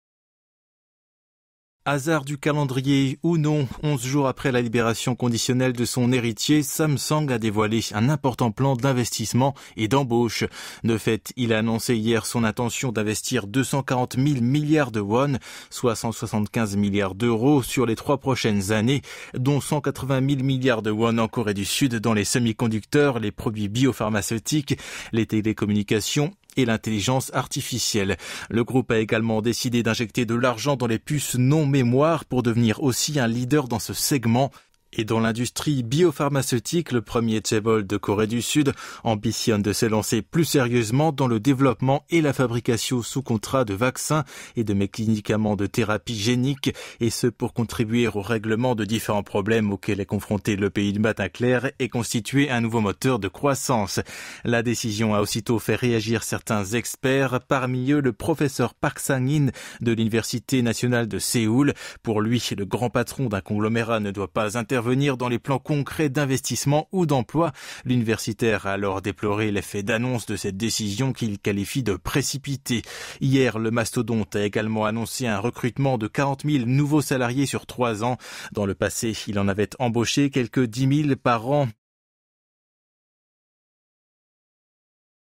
Samsung annonce un vaste plan d’investissement et de recrutement pour les ... | KBS NEWS 210825
Samsung annonce un vaste plan d’investissement et de recrutement pour les trois ans à venir